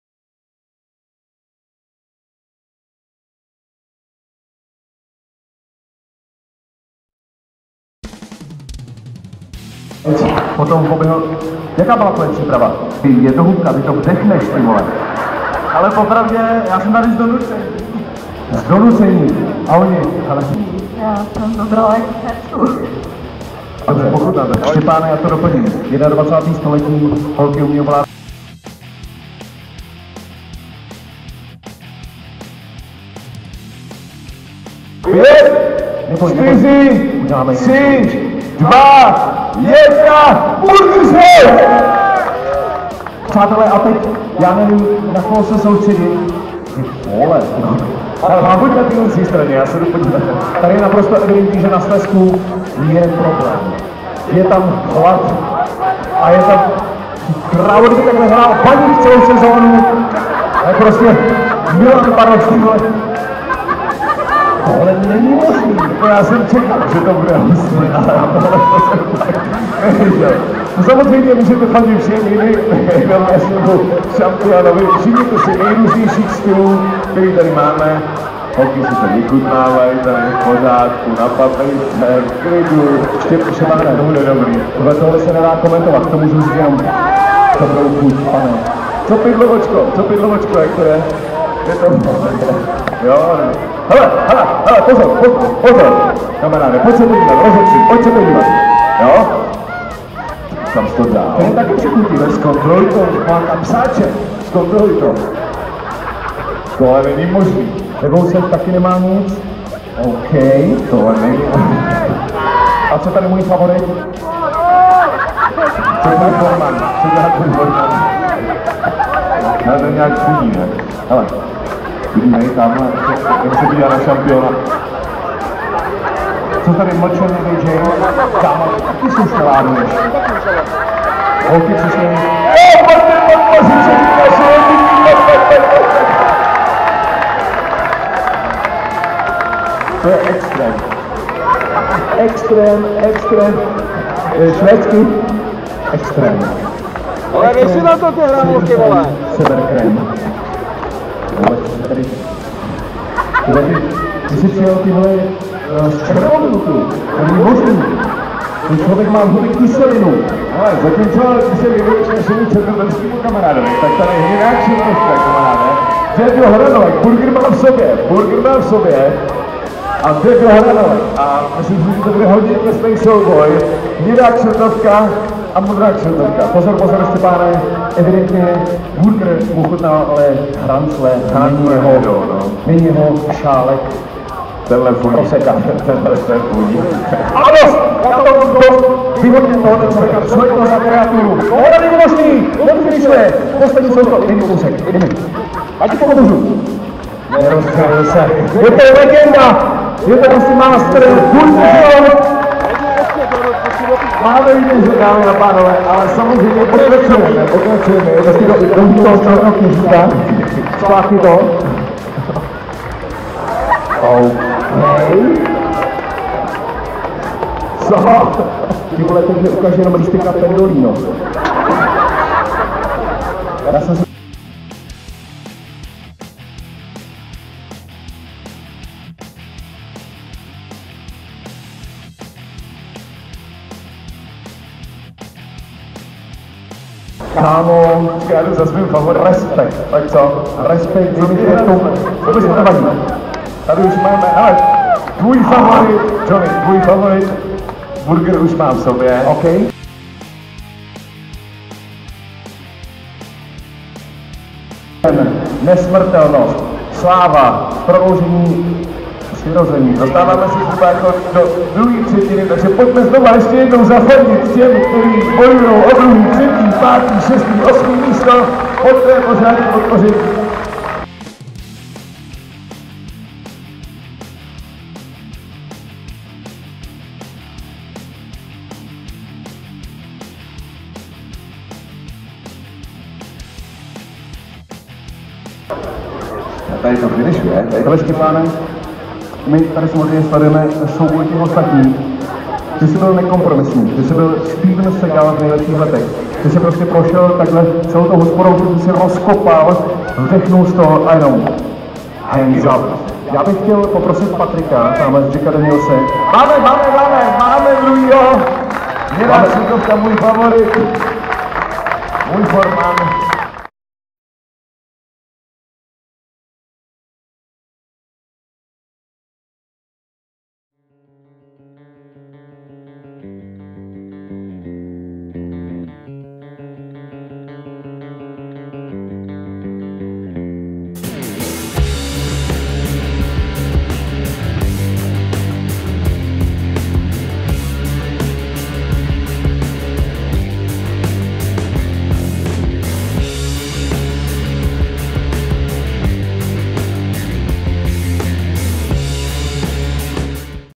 Burger and Fries Speed Eating Contest at Burgerfest, Prague w/ "Nibbler" Next to Me!!
This is a full contest footage of a Burger and Fries Eating Competition held at Burgerfest in Prague, Czech Republic. The first finisher wins a prize package including a camera, a bottle of Jack Daniel's and more. Watch the video to the end to see if I'm a winner and also a bonus in the form of "squirrel" lol.